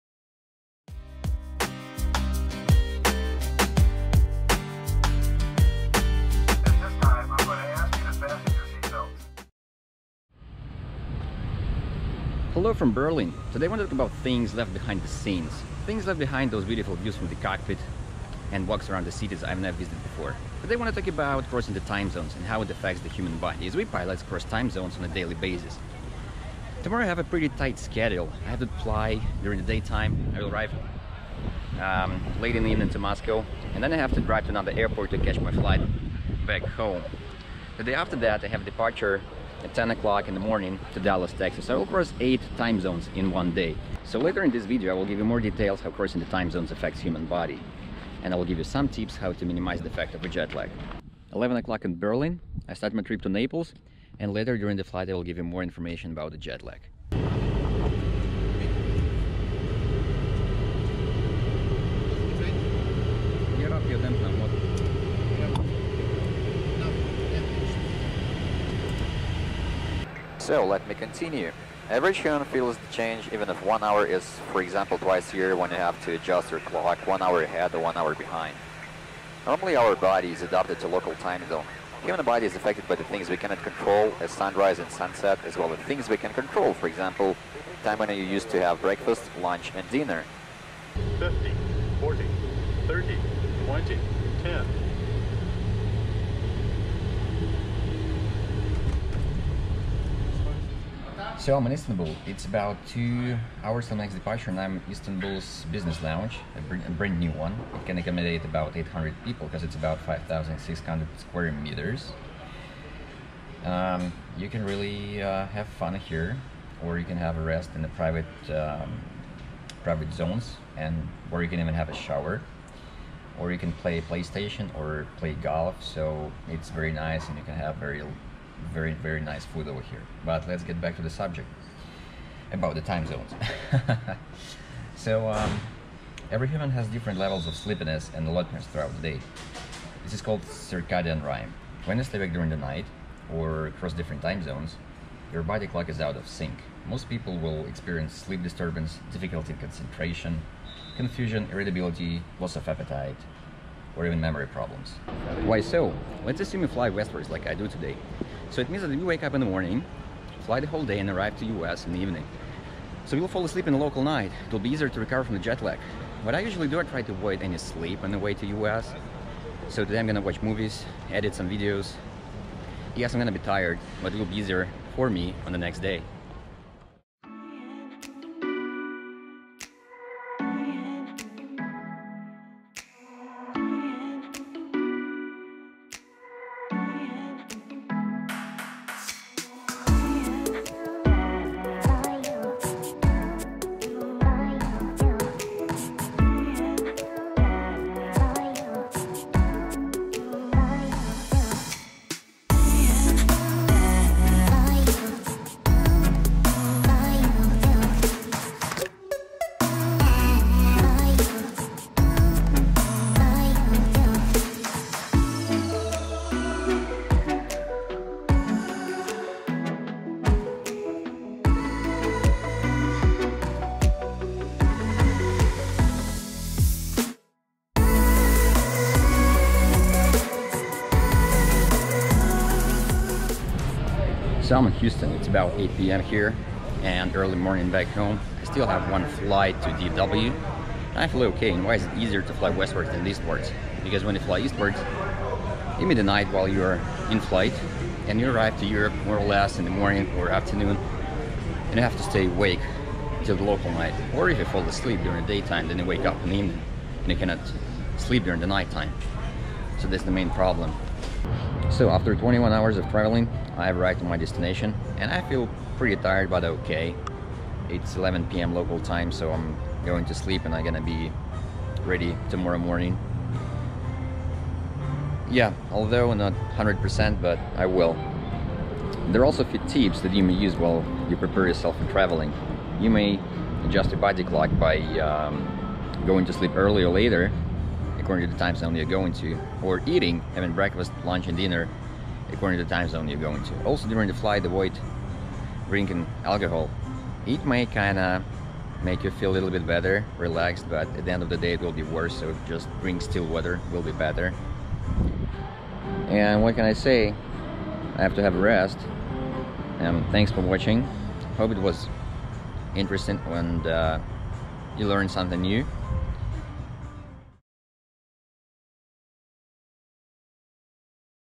How DOES JET LAG AFFECT human body? Explained by Pilot
How does jet lag affects human body? Why is it easier to travel westwards than eastwards? What is jet lag? Why do you feel jet lag and how to get over it?

I think most of us had a jet lag, and I as a pilot am exposed to jet lag almost on a daily basis. I would say changing few hours is not a big deal for us, however this affects regular people.

I will tell you what is jet lag and how to prepare yourself for travel and changing timezones on personal example as I will change 8 time zones within a day with a pretty busy schedule.

The only bonus I got was amazing Turkish Airlines service in Turkish Airlines Boeing 777 business class and Turkish Airplanes business class in brand new Istanbul airport.

Let me know if you like this kind of series.